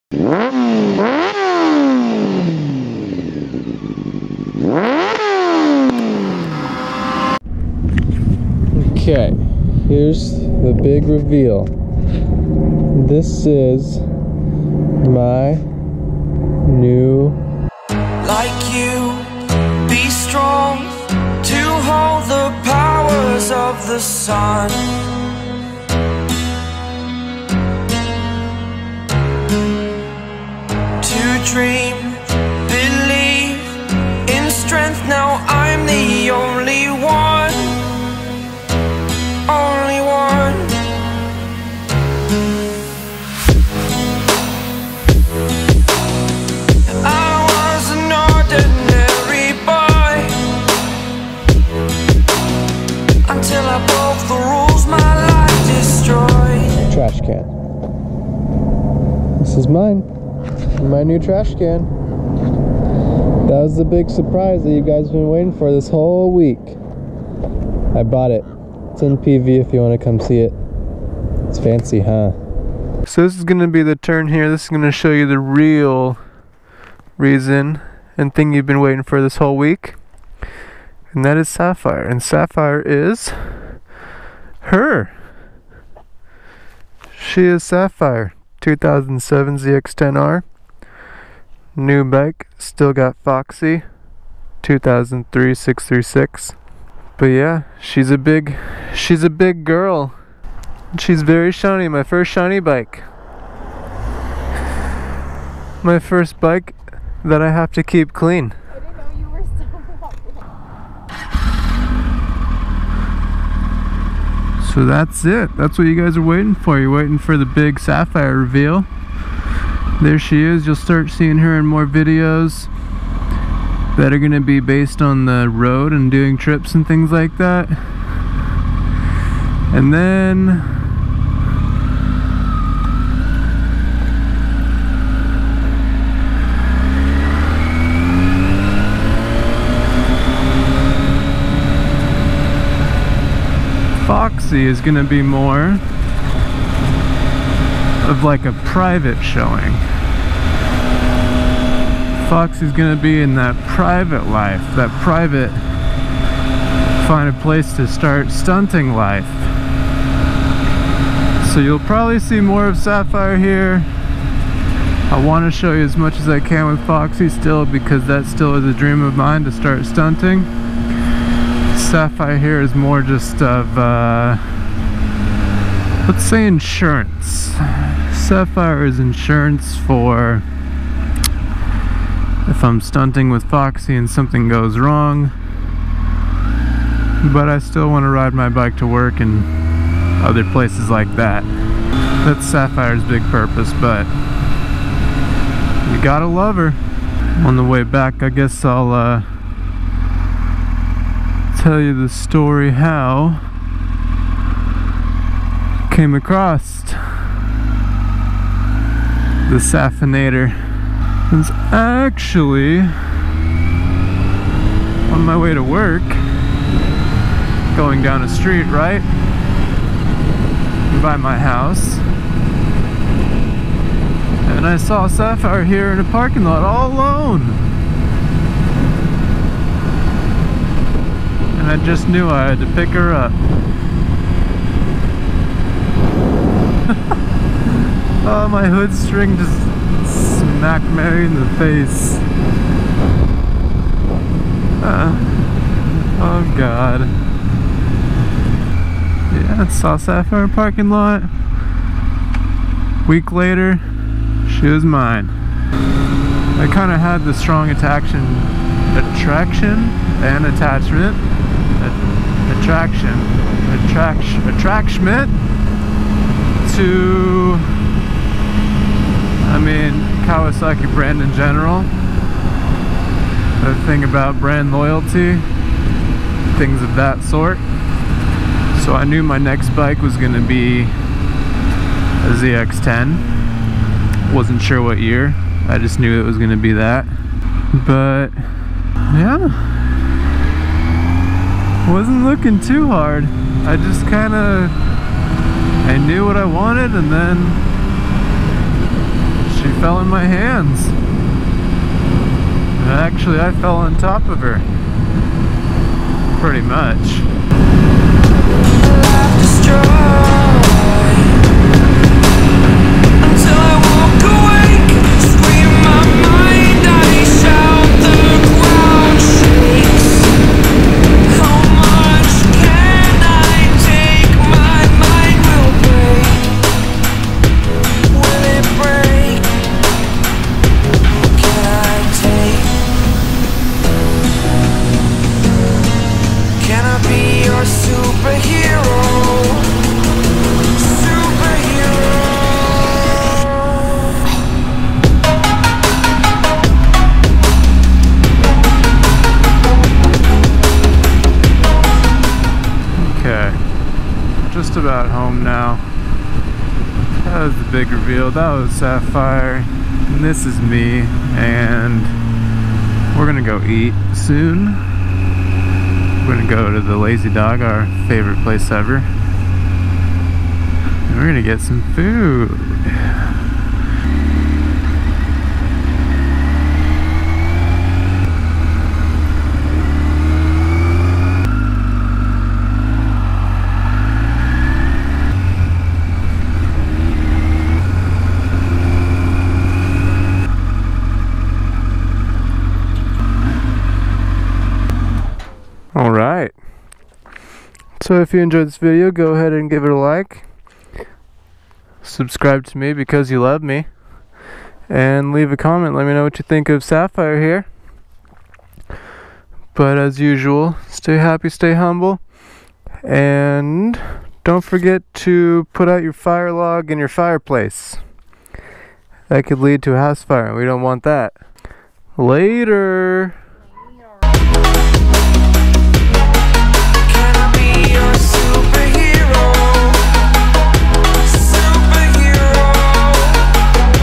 NEW BIKE REVEAL!!
In this video you finally get to see the new pride and joy...Sapphire!
Take a trip with me to unveil sapphire and lets hear how i managed to get this liter bike.